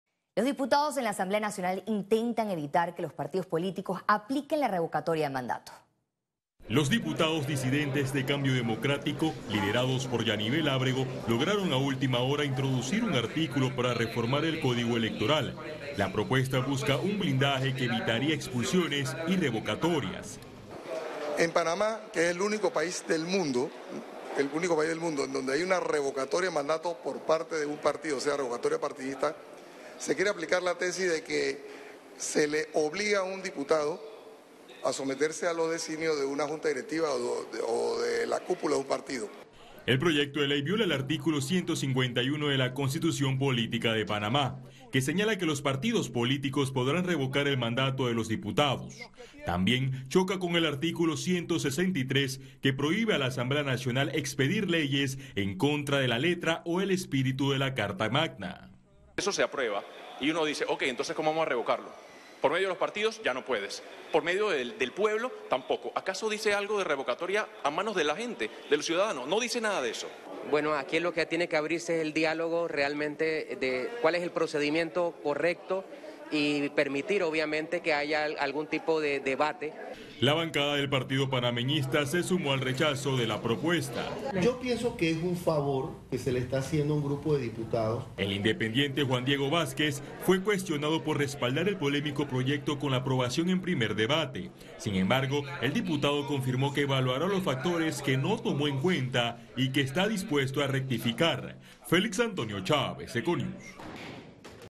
Diputados de la Asamblea Nacional reaccionan contra la propuesta de revocatoria | EcoNews
Panamá, 16 de marzo de 2022

Los diputados disidentes de Cambio Democrático, liderados por Yanibel Ábrego, lograron a última hora introducir un artículo para reformar el Código Electoral. La propuesta busca un blindaje que evitaría expulsiones y revocatorias.

El proyecto de ley viola el artículo 151 de la Constitución Política de Panamá, que señala que los partidos políticos podrán revocar el mandato de los diputados. También choca con el artículo 163, que prohíbe a la Asamblea Nacional expedir leyes en contra de la letra o el espíritu de la carta magna.
La bancada del Partido Panameñista se sumó al rechazo de la propuesta. Señaló que, “la propuesta busca establecer un ordenamiento en cuanto a la forma de las competencias de las jurisdicciones electorales, pero que la bancada del Cambio Democrático presentó un artículo delicado en cuanto al procedimiento de la revocatoria”.

El independiente Juan Diego Vásquez fue cuestionado por respaldar el polémico proyecto con la aprobación en primer debate, sin embargo, el diputado confirmó que evaluará los factores que no tomó en cuenta, y que está dispuesto a rectificar.